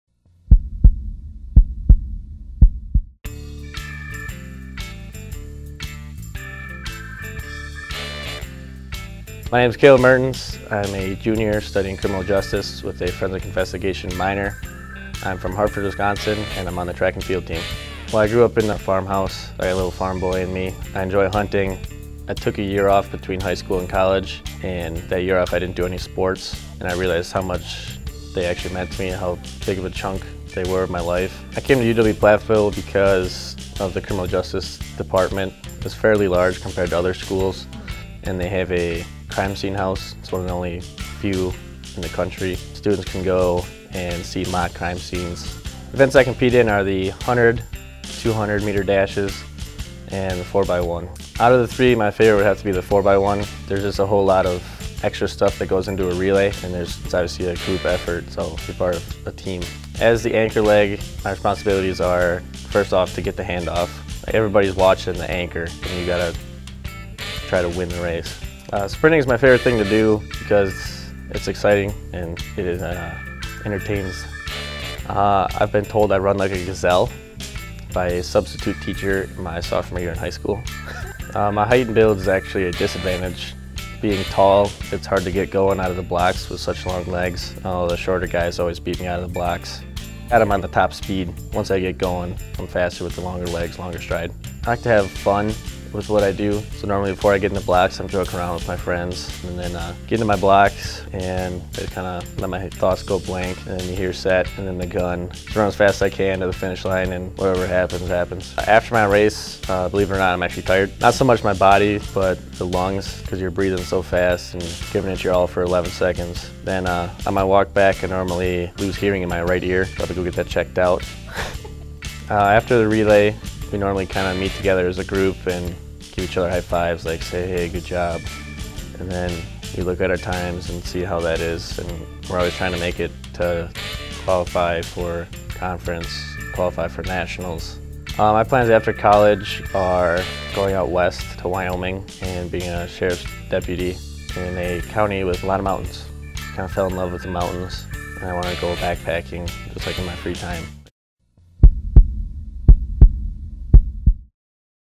Player Perspective Series | Caleb Mertens | Track & Field
Episode #22
Caleb Mertens | UW-Platteville Track and Field/Cross Country

Caleb Mertens, a member of the UW-Platteville Track & Field team from Hartford, Wisconsin. He is a junior majoring in criminal justice with a forensic investigation minor.

Letsgopioneers.com
Facebook.com/uwplatteville.athletics
YouTube.com/user/uwpPioneers

Producer, Editor: Ali Mercado
Production Assistant: Sarah Kolb